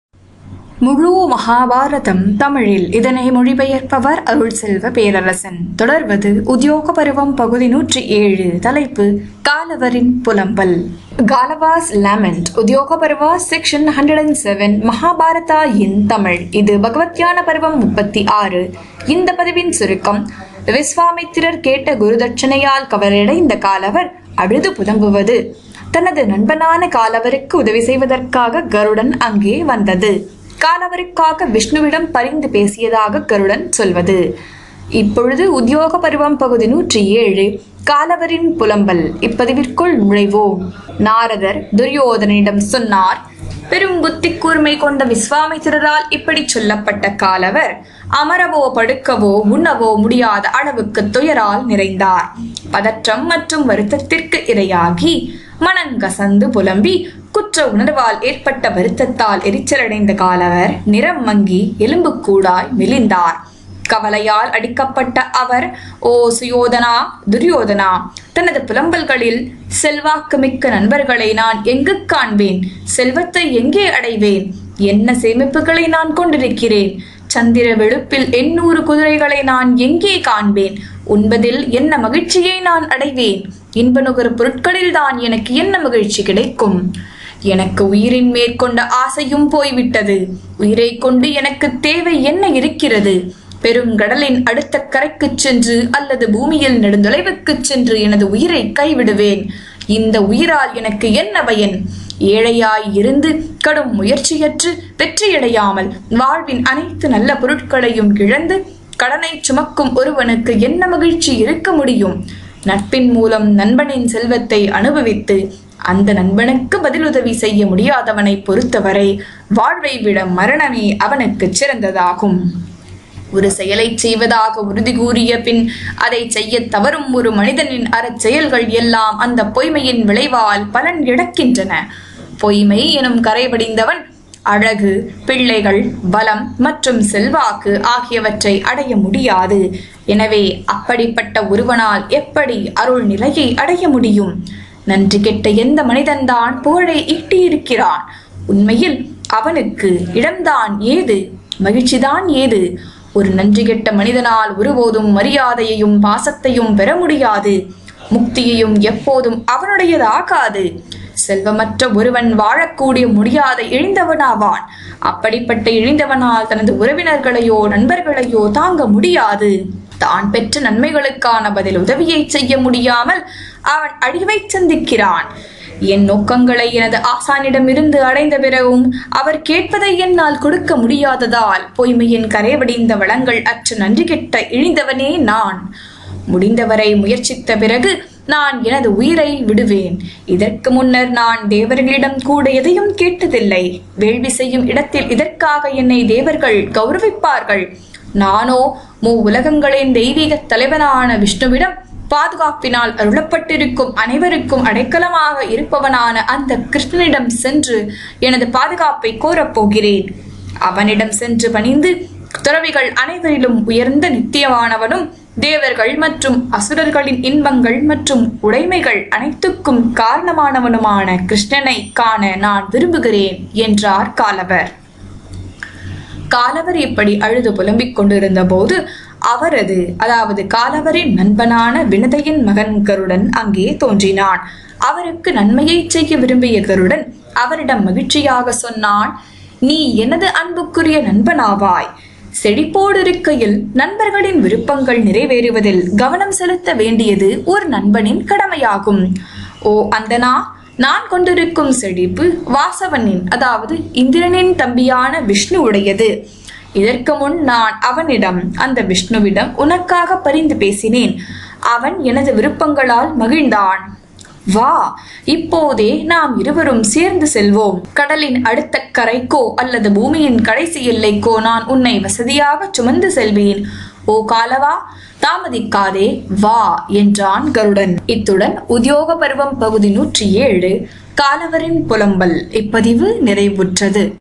Mahabharatham in Tamil | Udyoga Parvam - Section 107
முழு மஹாபாரதம் | உத்யோக பர்வம் - பகுதி 107

தலைப்பு : காலவரின் புலம்பல்!

குரல் : தேவகி ஜெயவேலன்
Narrated by : Devaki Jayavelan